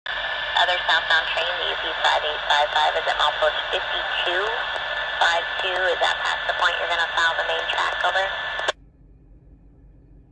UP 5855 milepost location
Was waiting in Whiting when my train scanner going off with the dispatcher indicating the location of a southbound Union Pacific mixed freight along the Sioux City Subdivision. Based on what I heard the train was about 6 miles north of where I'm at and heading south to my location. I ended up catching this train in Whiting about 10 minutes later.